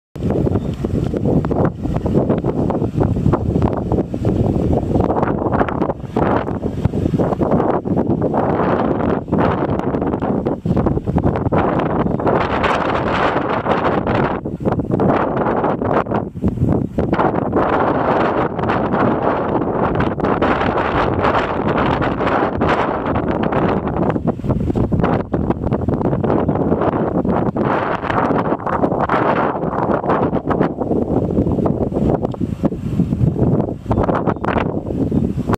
2008 Dodge Grand Caravan SE van for sale at auction | bidding closes January 26, 2022
2008 Dodge Grand Caravan SE  van
 Miles: 193,909 on odometer
 VIN: 2D8HN44HX8R771841
Engine
 Displacement: 3.3L V6 OHV 12V
 Cylinders: 6
 Fuel type: Gas
Transmission
 Automatic
Interior
 AC, Heat
 Power windows, Power locks
 Cruise control
Tires
 Size: 225/65R16
Notes
 Hail damage
Oklahoma salvage title
It's located in Kingfisher, OK, and we're selling it to the highest bidder regardless of price.